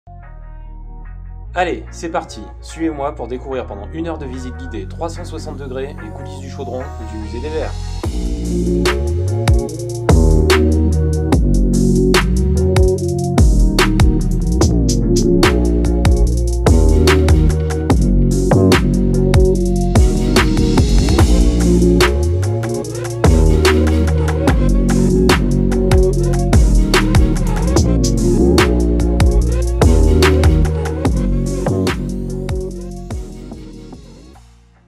Profitez d'une visite guidée en ligne du Chaudron et du Musée
Contrainte de suspendre les visites du stade Geoffroy-Guichard et du Musée des Verts durant la crise sanitaire, l’ASSE est heureuse de proposer à ses supporters une toute nouvelle expérience de visites interactives et immersives à 360°. Ces visites du Chaudron et du Musée des Verts s'effectueront en compagnie d’un guide durant près d’une heure lors d’une session en petit comité.
Installés devant l'écran de leur ordinateur, les visiteurs suivront le guide et pourront lui poser des questions grâce au chat développé sur l’application ou même par l’intermédiaire de leur micro. Cette expérience inédite repose sur la diffusion d'une vidéo captée en 360° dans le Chaudron et le Musée des Verts, complétée par un dispositif favorisant l’interactivité, audio et texte, en temps réel.
Accueil sur le parvis de Geoffroy-Guichard, entrée des joueurs, vestiaire de l'ASSE, tunnel d’entrée sur la pelouse, bord de terrain, salons, zone mixte et salle de presse : voici autant de lieux qui sont proposés au cours de la visite du stade. La visite du Chaudron sera par ailleurs agrémentée de témoignages de Loïc Perrin, ancien capitaine emblématique des Verts, et de Ninon Blanchard, joueuse à l’ASSE depuis 2013.